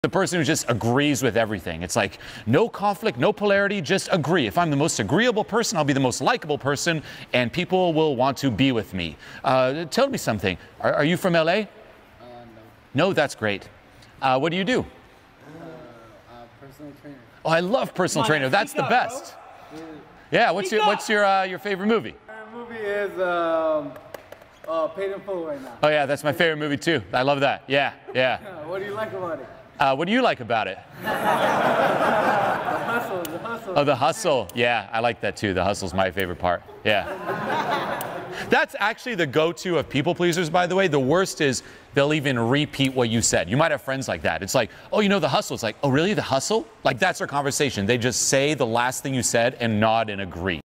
This Is Why You’re A People Pleaser! ⚠️
Here's what happens when you're a people pleaser... And what you can do about it!

Julien Blanc (AKA JulienHimself) is a Swiss-born, U.S.-based self-help speaker, entrepreneur and transformational coach.

What causes people pleasing and how to stop being a people pleaser! In this short, Julien Blanc (AKA Julien Himself) talks about people pleasing behavior and how to stop people pleasing.